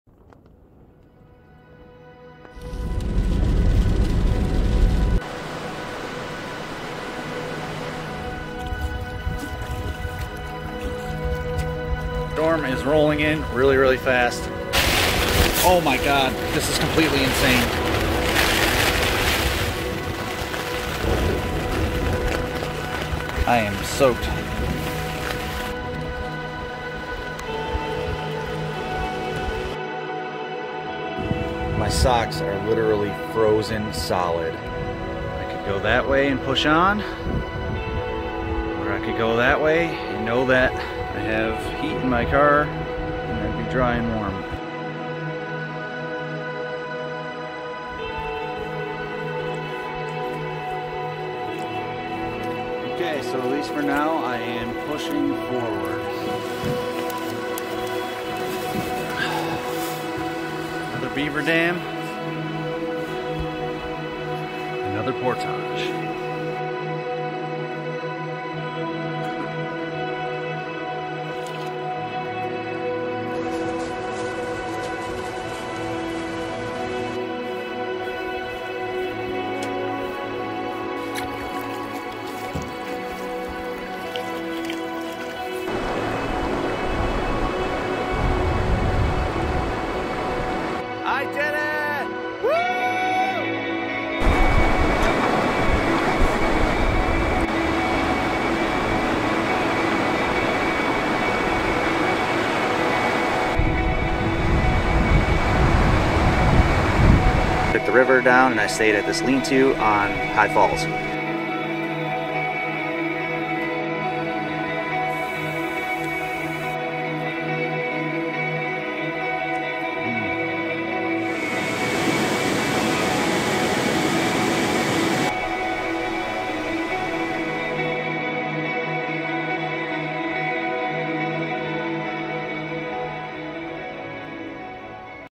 Oswegatchie River 3 Day Solo Canoe Trip - Trailer
On October 7th, I headed out alone to the Adirondacks for a 3 day canoe trip on the Oswegatchie River in Wanakena, NY. Storms hit on Day 1 and with wet gear and freezing temperatures I considered leaving after the first day. However, I carried on and carried over many beaver dams. After many miles paddling I made it to my destination of High Falls. I cooked up a rack of lamb on an open fire and stayed the night in an Adirondack lean-to. On day 3, the weather was perfect and I had an enjoyable paddle back out down river to Inlet.

Full video to come later!

Keep staying positive in 2021!
-Mark